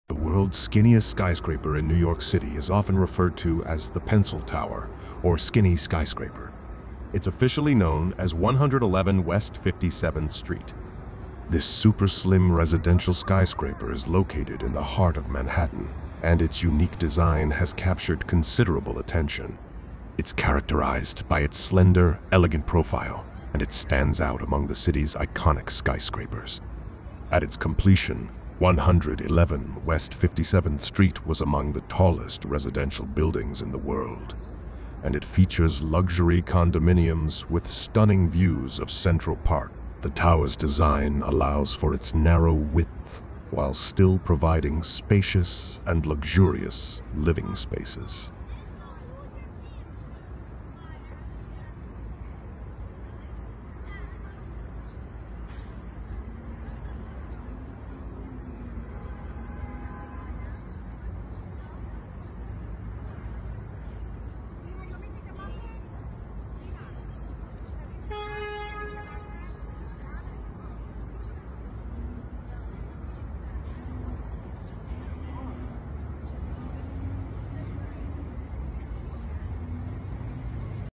The World's Skinniest Skyscraper in New York City
Movie Mistakes Analyzer | the world's skinniest skyscraper in New York City is often referred to as the "Pencil Tower" or "Skinny Skyscraper." It's officially known as 111 West 57th Street.
This super-slim residential skyscraper is located in the heart of Manhattan, and its unique design has captured considerable attention. It's characterized by its slender, elegant profile, and it stands out among the city's iconic skyscrapers.
At its completion, 111 West 57th Street was among the tallest residential buildings in the world, and it features luxury condominiums with stunning views of Central Park. The tower's design allows for its narrow width while still providing spacious and luxurious living spaces.